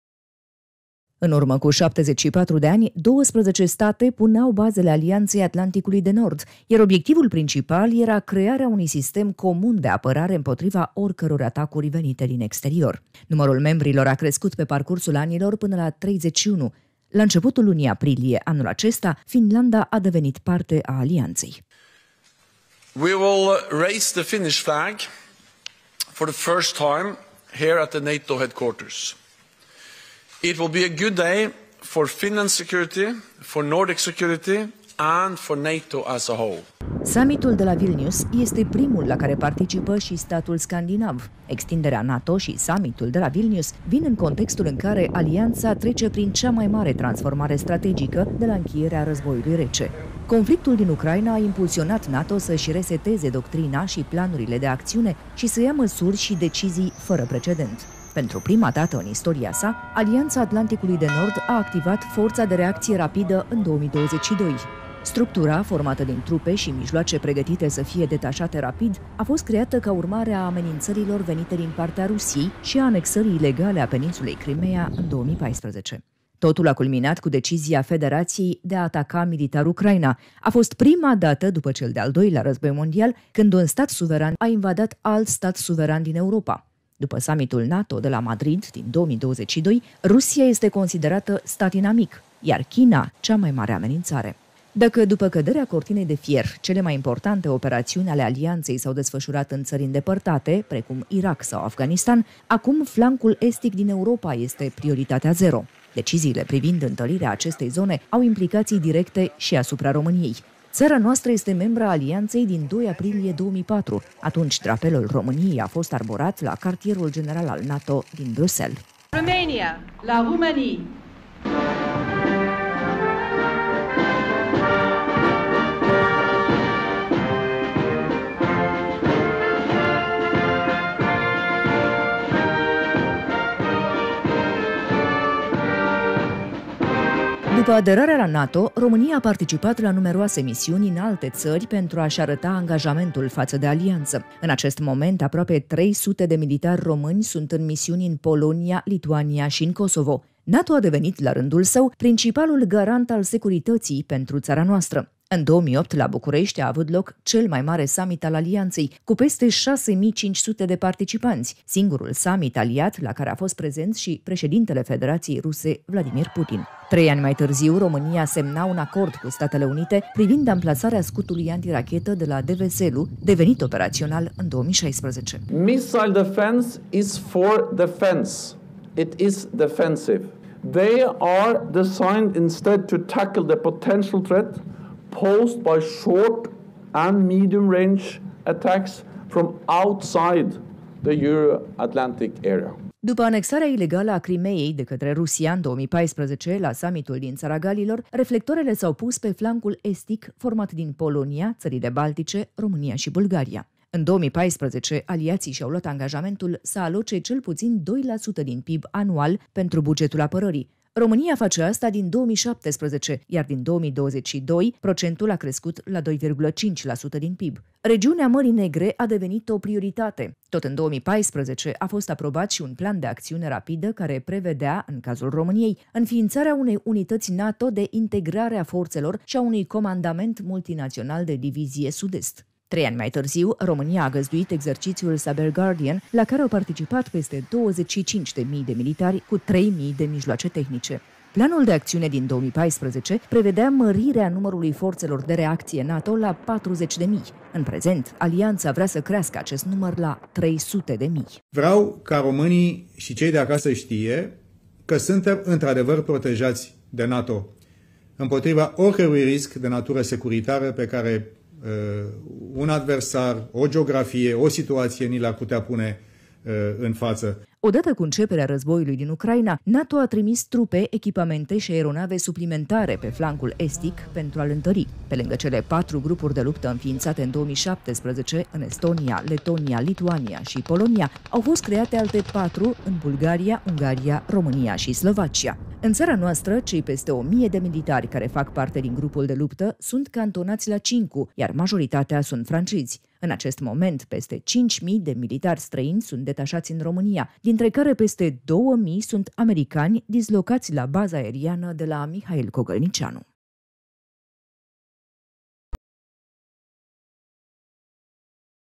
Cum a transformat războiul din Ucraina Alianța Nord-Atlantică
Războiul din Ucraina a schimbat complet mediul de securitate din Europa și a transformat doctrina strategică a NATO, anul trecut fiind cel mai complex din istoria Alianței. Rusia a devenit din partener - dușmanul Alianței, iar China cea mai mare amenințare.